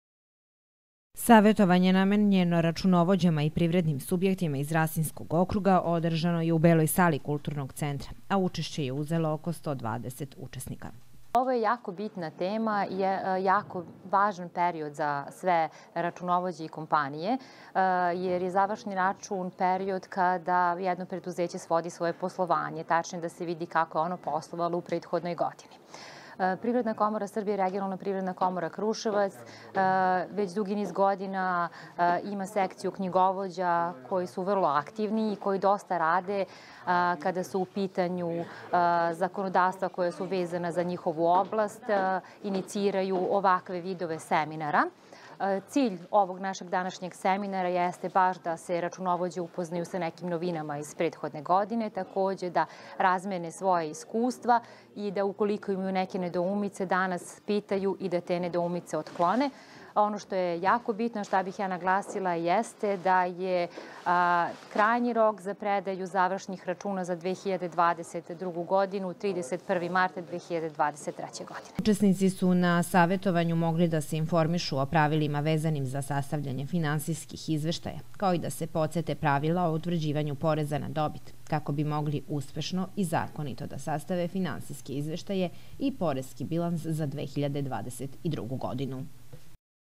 ODRŽANO SAVETOVANJE O SASTAVLJANJU I PREDAJI FINANSIJSKIH IZVEŠTAJA ZA 2022. GODINU
Regionalna privredna komora Kruševac u saradnji sa kompanijom Paragraf organizovala je savetovanje na temu „Sastavljanje i predaja finansijskih izveštaja i utvrđivanje poreza na dobit za 2022. godinu“.